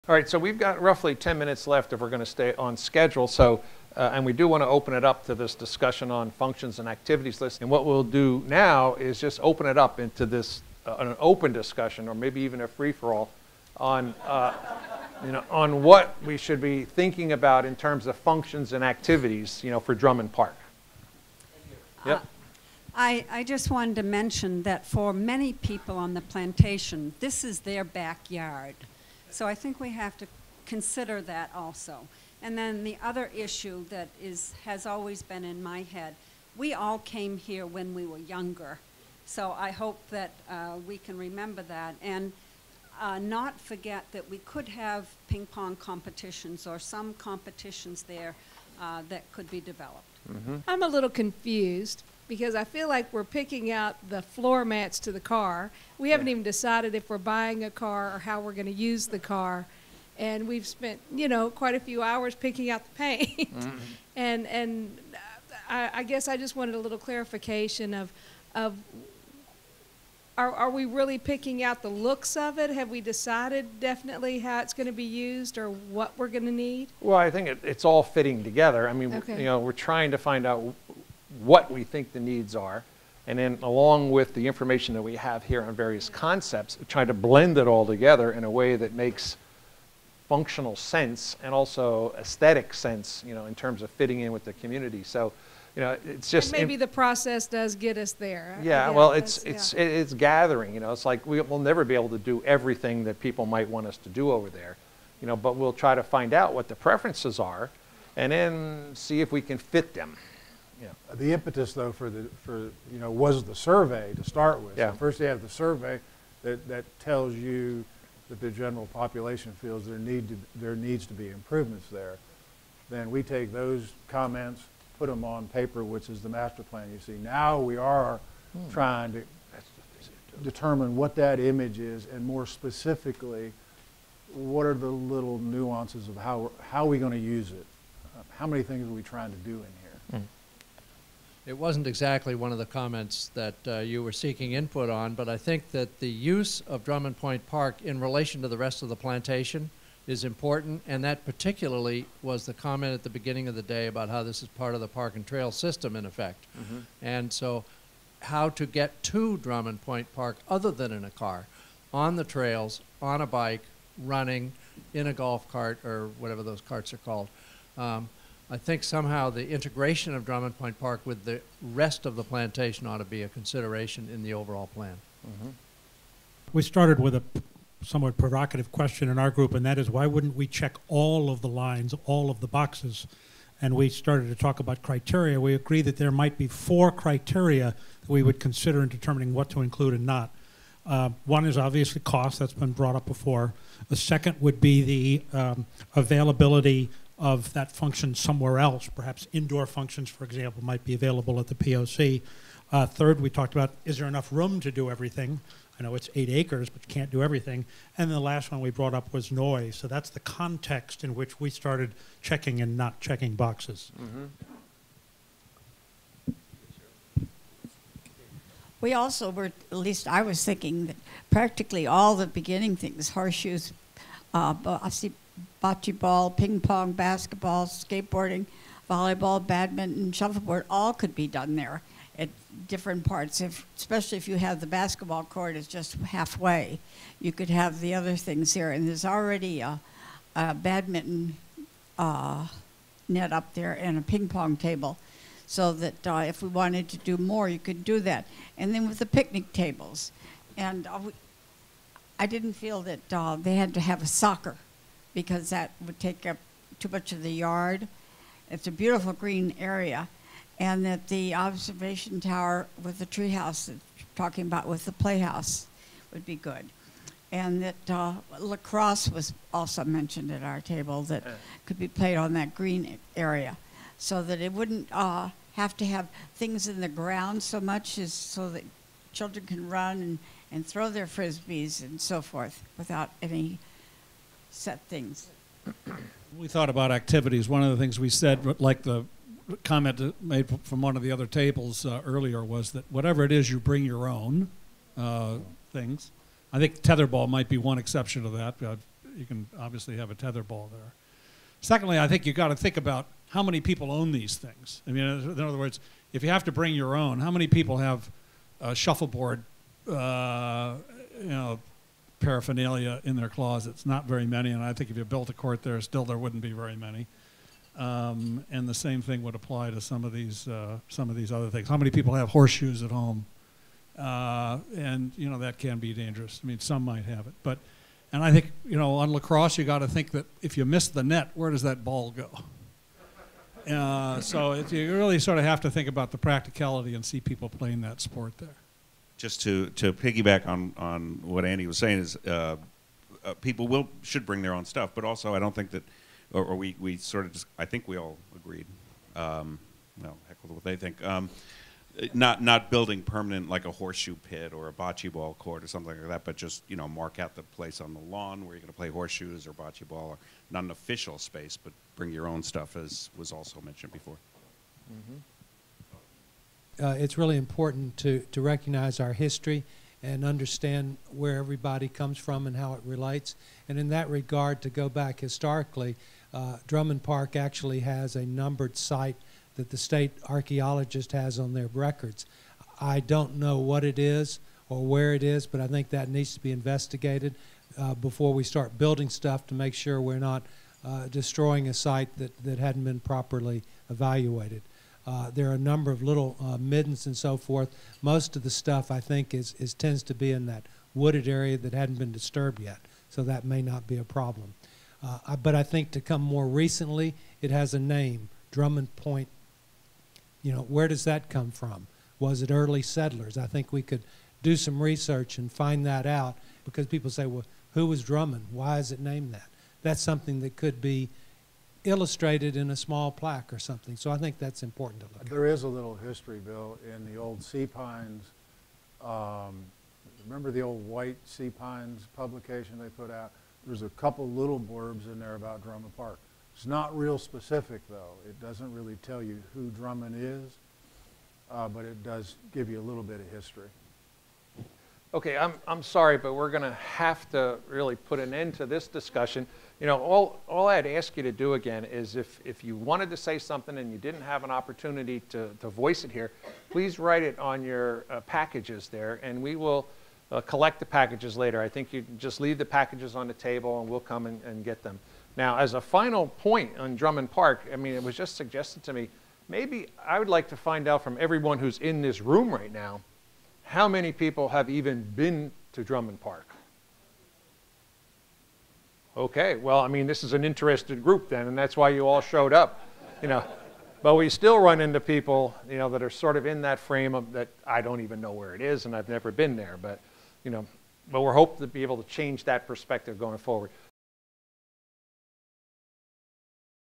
Drummond Point Park: Checklist 4/26/13
The Amelia Island Plantation Community Association held a Town Hall meeting to explore the use and improvement of the Drummond Point Park amenity. Led by Joe Bunting, Judith Pines and Steve Kaye, the interactive workshop was well attended by interested property owners of the coastal resort community. The development project is being led by architect John Cotner, who presented concepts at the meeting.